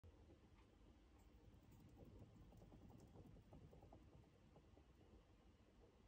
Female tarantula feels a male in the room - Gets EXCITED 😏
2nd channel :
~ Beyond the Lair

I was in the middle of editing a video when I heard some loud rapid tapping. Was wondering what it was. It kept happening every 5 mins or so.. Then I realized it was my female Lasiodora klugi (Bahia Scarlet). She probably detects a matured male in the room somewhere xD